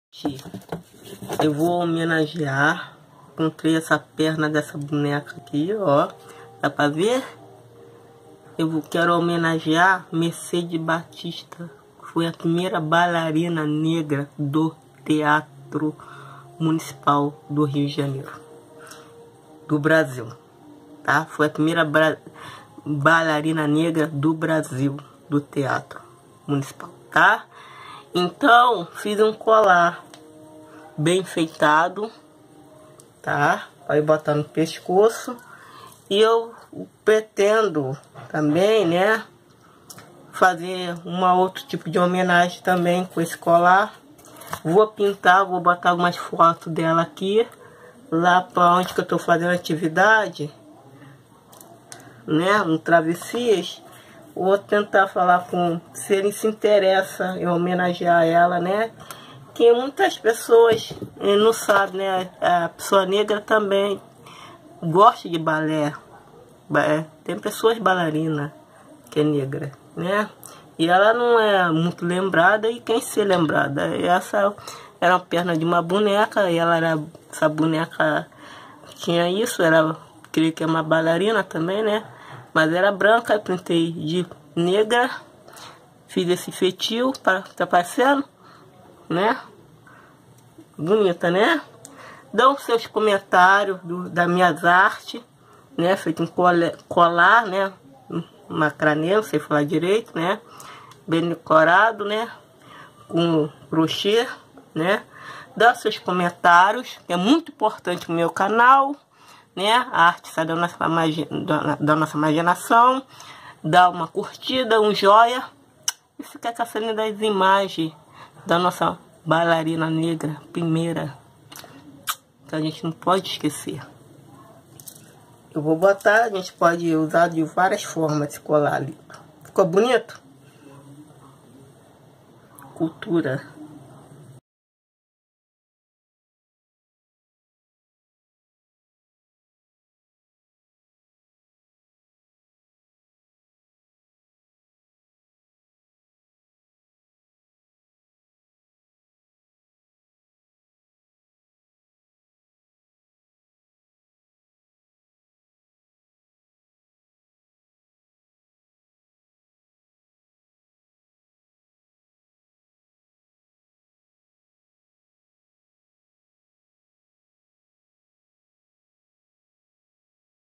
Renata Alves De Mello, mostra o colar para Homenagear a primeira bailarina negra do Brasil.
artesanato de reciclagem, Homenagem a primeira bailarina negra do Brasil, reaproveitar o próprio lixo transformar numa arte de imaginação, reproduzir, construir, fazer,arte de expressão.